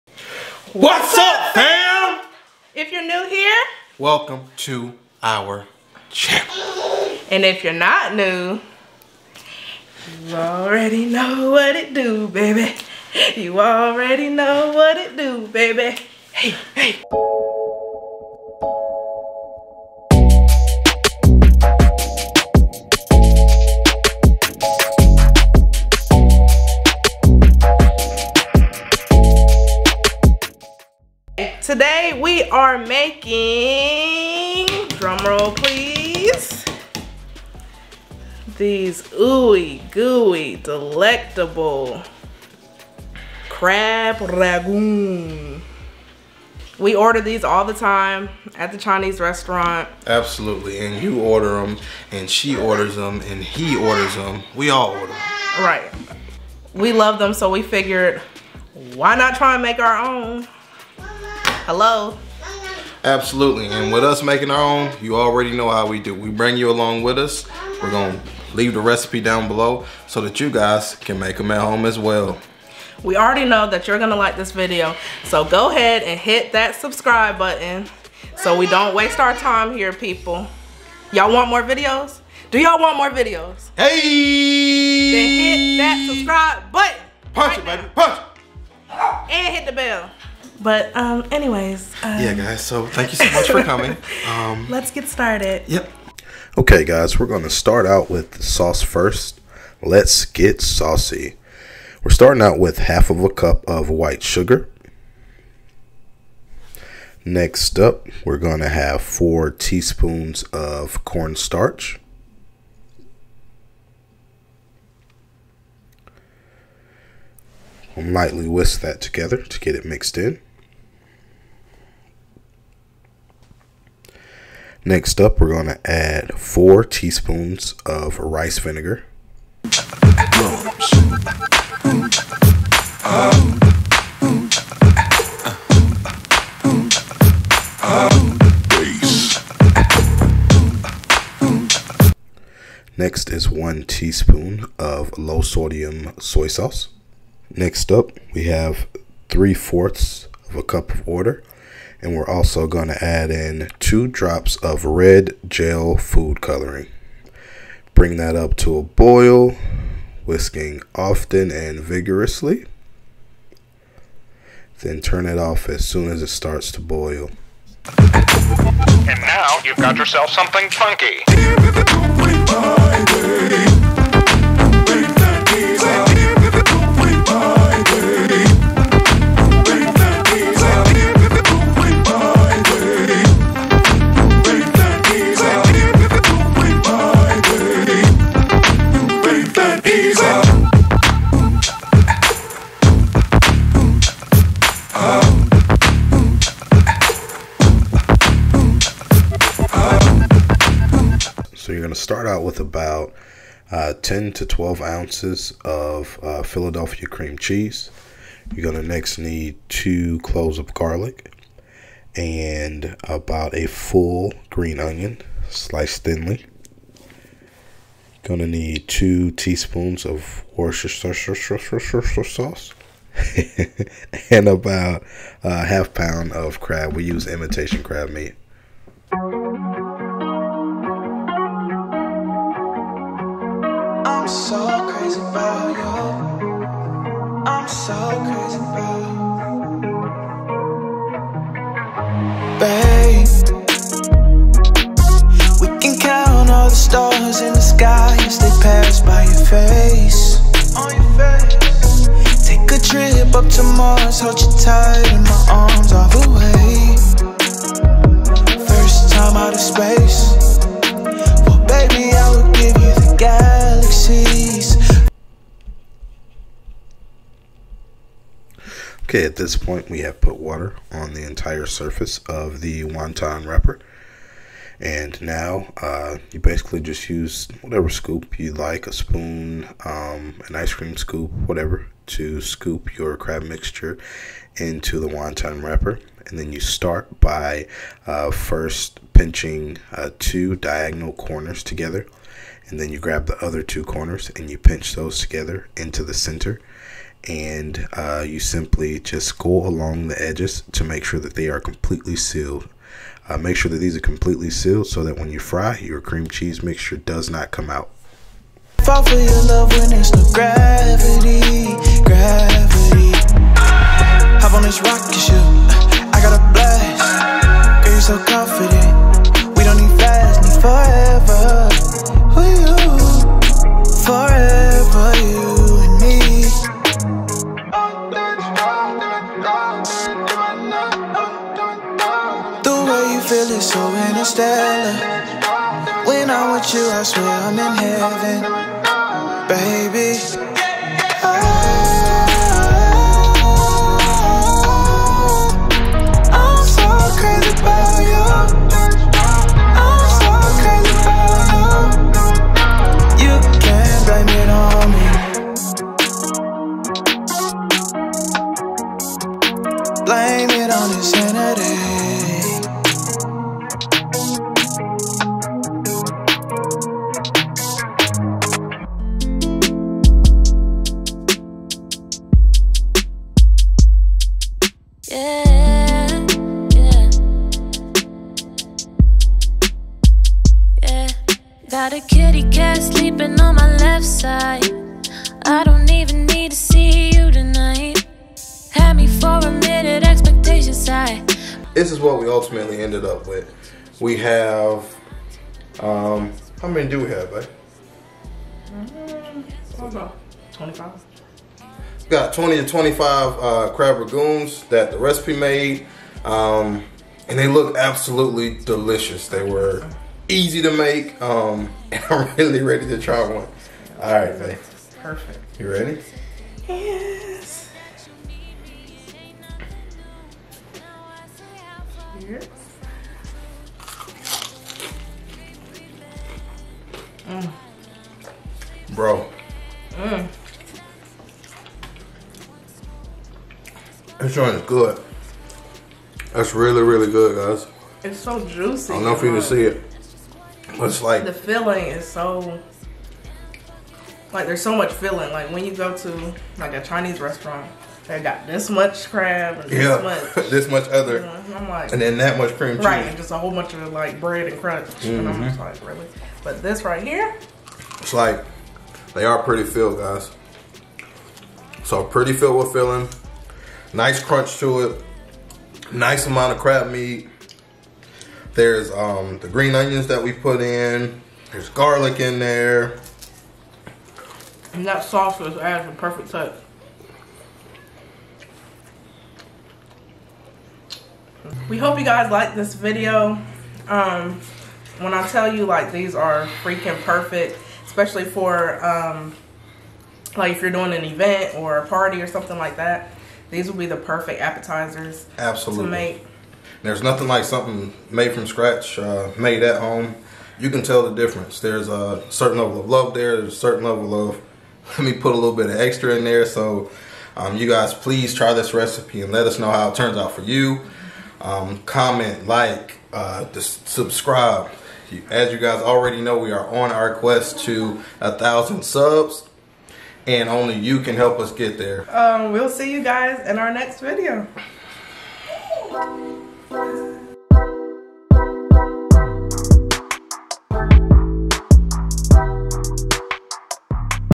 Super Easy Restaurant Style Crab Rangoon | Crab and Cream Cheese Wontons
We are making crab ragoon! This recipe is perfect for a party or event appetizer. This is a Chinese restaurant style dish you can make at home with just a few ingredients.

Our spice shop!!